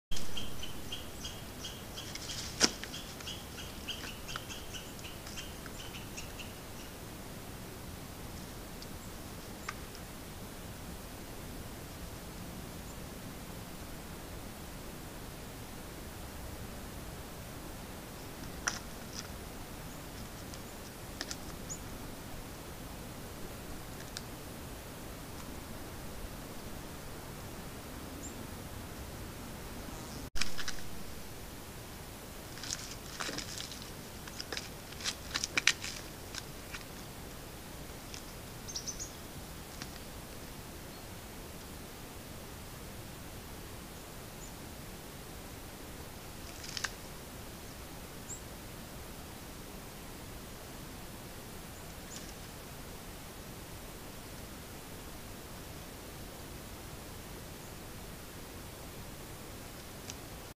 Golden Eagle Feeding on a Bull Elk Carcass
Big bird alert! The size of this golden eagle is astounding. It dwarfs the bull elk carcass that it is scavenging on. This footage was captured in Utah using a Browning trail camera.